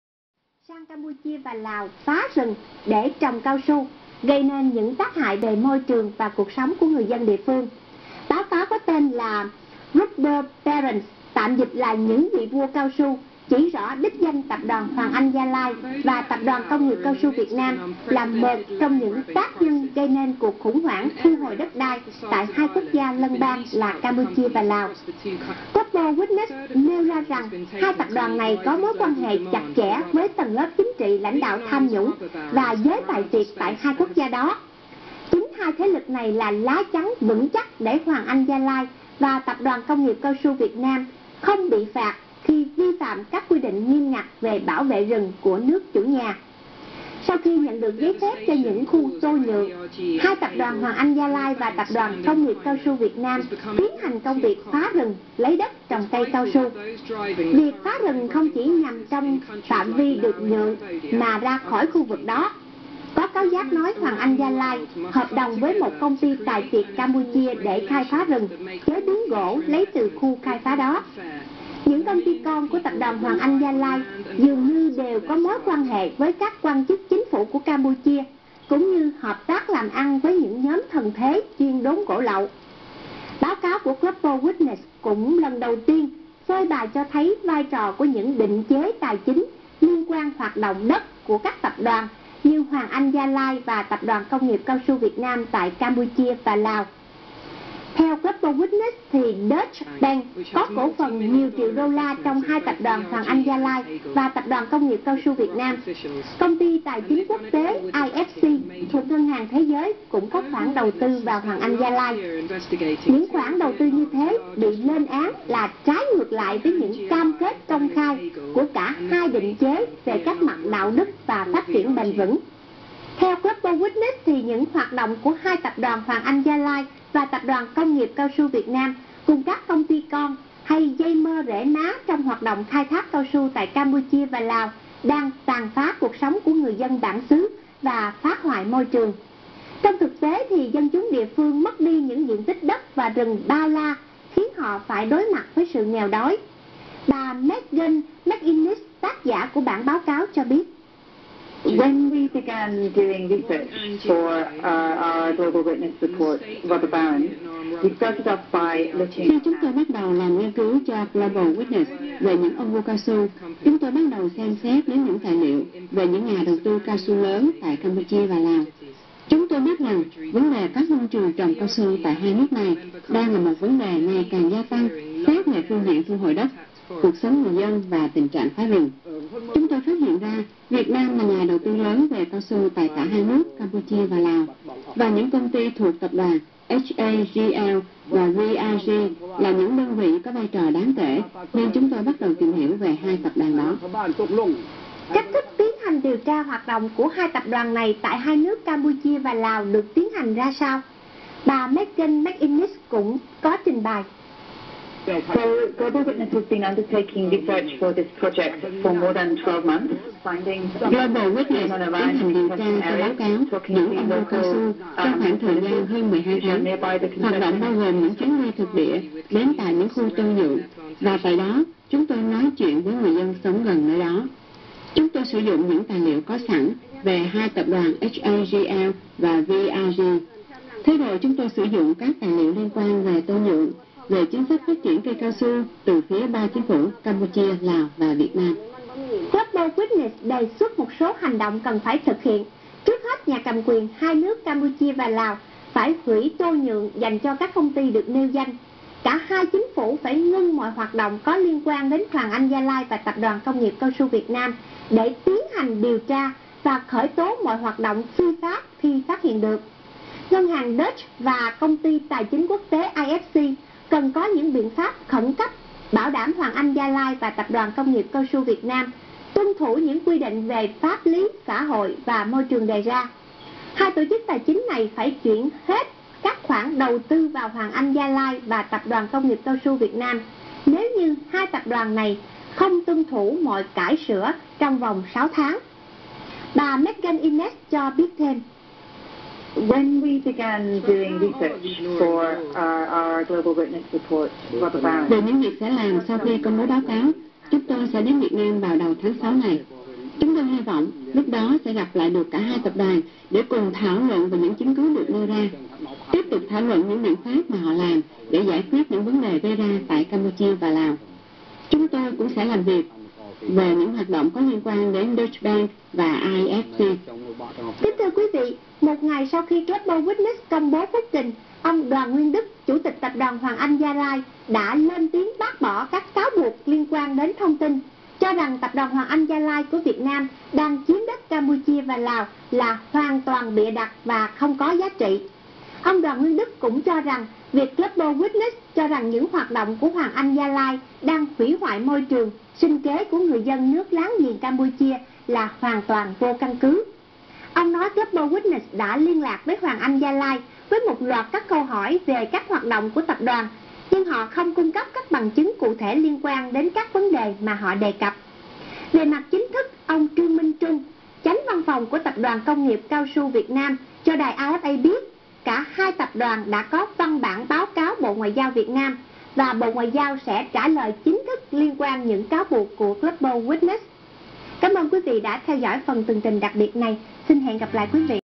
Laos and Cambodia Governments to immediately cancel the rubber concessions in Requested
About the report

This report shows how vast amounts of land have been acquired for rubber plantations in Cambodia and Laos by two of Vietnam's biggest largest companies, Hoang Anh Gia Lai (HAGL) and the Vietnam Rubber Group (VRG). The rubber barons are financed by international investors including Deutsche Bank and the International Finance Corporation (IFC) -- the private lending arm of the World Bank.

It lays bare the culture of secrecy and impunity that has allowed these two rubber giants to gain rights to more than 200,000 hectares of concession land through secretive deals with the Lao and Cambodian governments. They have close links with the region's corrupt political elites and operate with complete impunity, devastating local livelihoods and the environment in the process. Rubber Barons is the first exposé of the role of international financiers in these land grabs. Deutsche Bank has multi-million dollar holdings in both companies, while the IFC invests in HAGL.

Global Witness urges the governments of Laos and Cambodia to immediately cancel the rubber concessions in question, suspend and investigate VRG and HAGL's rubber operations, and where there is evidence of illegal activities, prosecute the companies. It also calls on international governments to bring in and enforce regulations to stop international banks and financial institutions from continuing to turn a blind eye to deforestation and human rights abuses.

When approached by Global Witness in advance of publishing Rubber Barons, HAGL confirmed holding rubber plantations totalling 46,752 hectares in Cambodia and Laos but denied knowledge of any disputes with local communities or their involvement in illegal activities. VRG meanwhile stated the evidence presented to them was not true, but declined to confirm the status or holdings of its rubber operations in either country.

Following the publication of this report, Hoang Anh Gia Lai (HAGL) CEO Doan Nguyen Duc held a press conference during which the company contested our evidence.